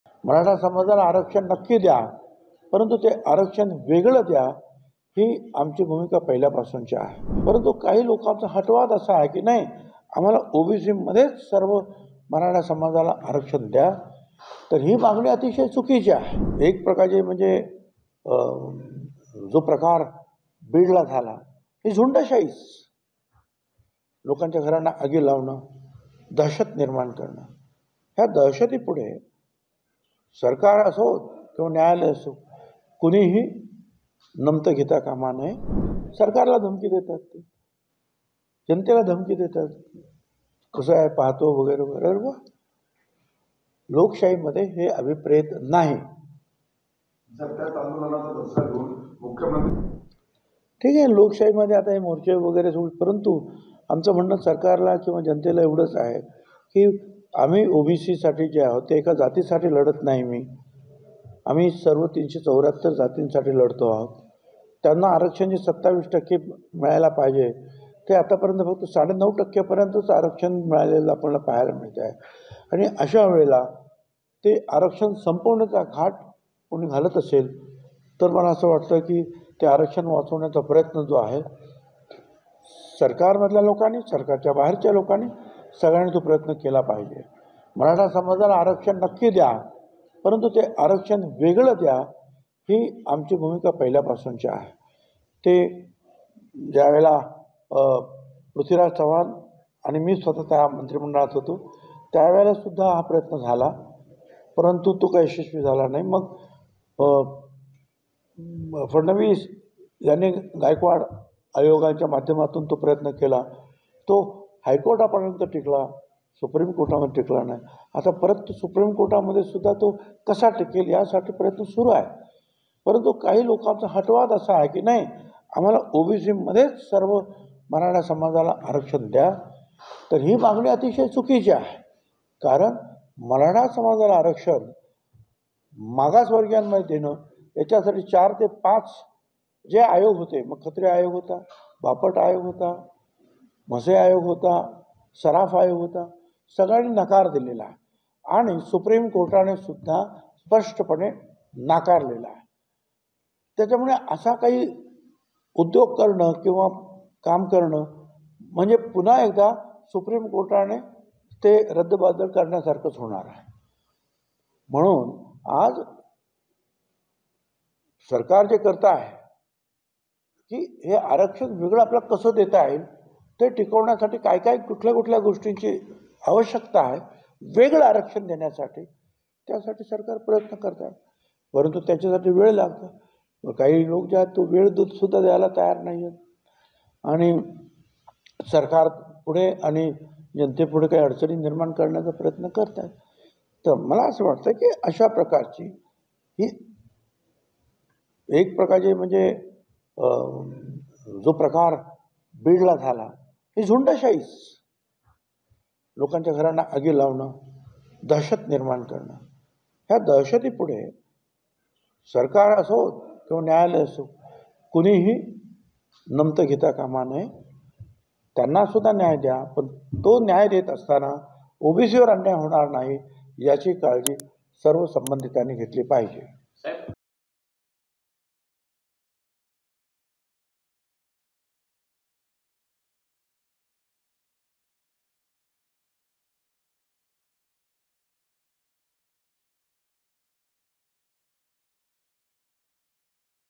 Chhagan Bhujbal on Jarange Patil : मंत्री छगन भुजबळांनी पुन्हा डिवचलंच... | LetsUpp Marathi
मराठा समाजाला ओबीसीतून आरक्षण देण्याची मागणी चुकीचीच असल्याचं पुन्हा एकदा अन्न व नागरी पुरवठा मंत्री छगन भुजबळांनी सांगितलं आहे.